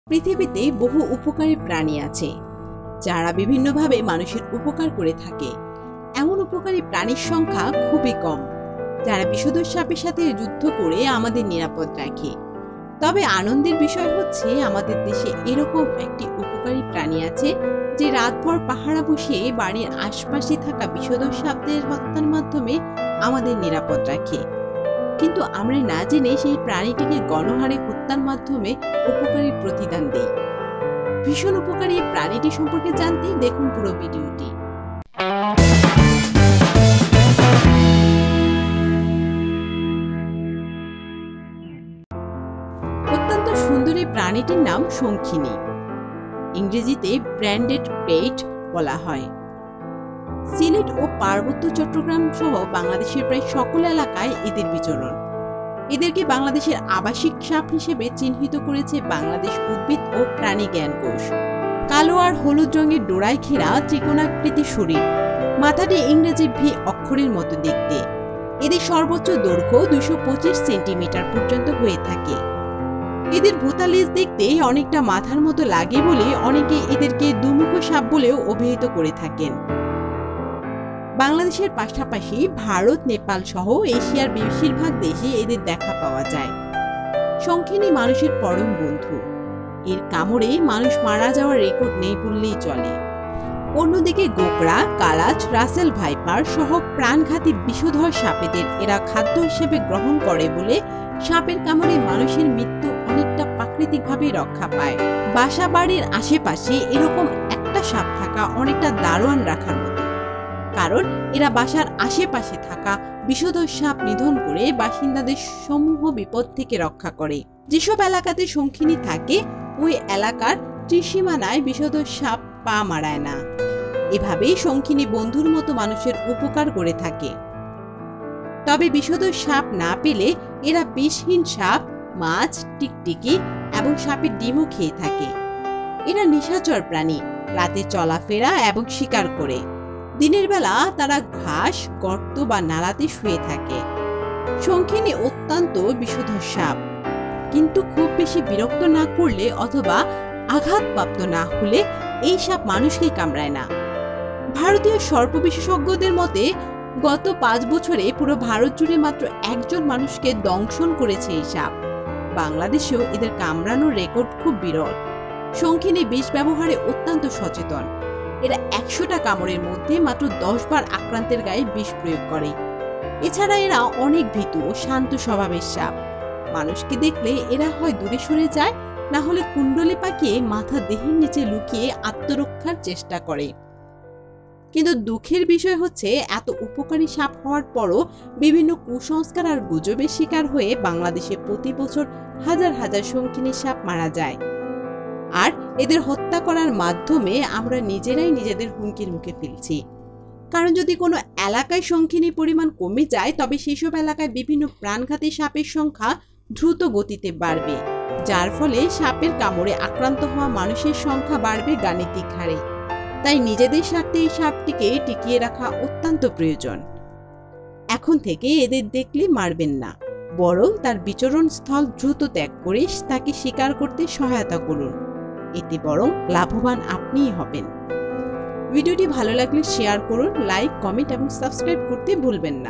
পাহারাদার সাপ : শঙ্খিনী যেভাবে মানুষের জীবন বাঁচায় l বটতলা - Banded Krait ‍ূSnake
অত্যন্ত সুন্দর এই সাপটির নাম শঙ্খিনী, ইংরেজীতে  Banded Krait

শঙ্খিনী সাপ মানুষের পরম বন্ধু । এর কামড়ে মানুষ মারা যাওয়ার রেকর্ড নেই বললেই চলে ।

অন্য দিকে গোখরা. কালাচ, রাসেলস ভাইপার সহ প্রাণঘাতি বিষধর সাপদের এরা  খাদ্য হিসেবে গ্রহন করে বলে সাপের কামড়ে মানুষের মৃত্যু অনেকটা প্রাকৃতিকভাবেই রক্ষা পায়।

বাসাবাড়ির আশপাশে শঙ্খিনী সাপ থাকা অনেকটা দারোয়ান রাখার মত,  কারন,   যেসব এলাকাতে শঙ্খিনী বা Banded Krait সাপ থাকে অই এলাকার ত্রি সীমানায় বিষধর সাপ পা মাড়ায় না।

ভয়ানক ও বিরল প্রজাতির এই সাপ দেখলে অবাক হবেন আপনিও।

 আজব এই প্রাণীটির সম্পর্কে মজার সব তথ্য জানতে দেখুন পুরো ভিডিওটি।

লাইক, কমেন্ট ও শেয়ার করে বটতলার সাথে থাকুন।